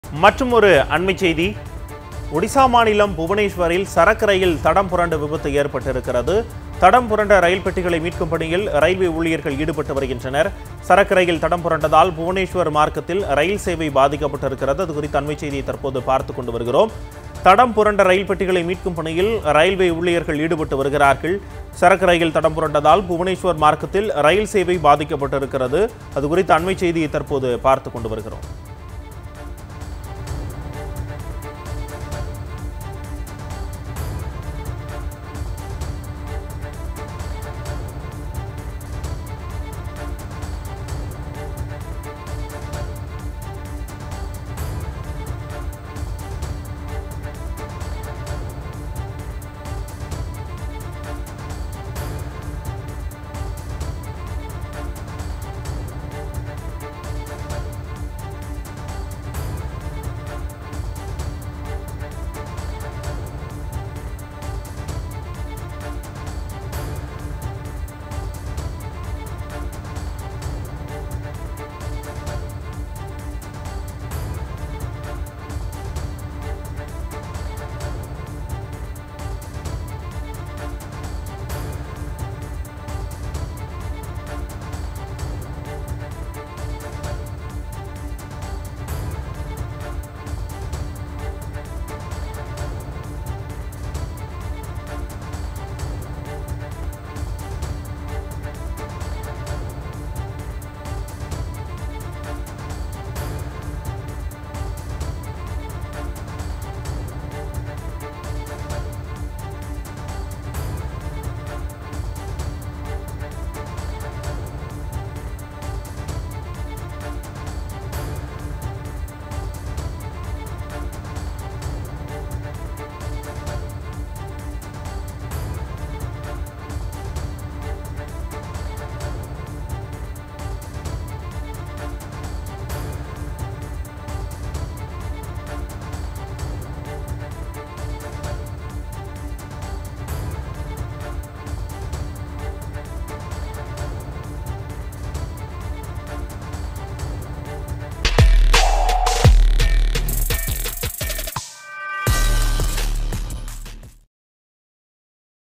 BREAKING | ஒடிசாவில் சரக்கு ரயில் தடம் புரண்டு விபத்து! | Odisha | TrainAccident
ஒடிசாவில் சரக்கு ரயில் தடம் புரண்டு விபத்து! | Odisha | TrainAccident | Kalaignar News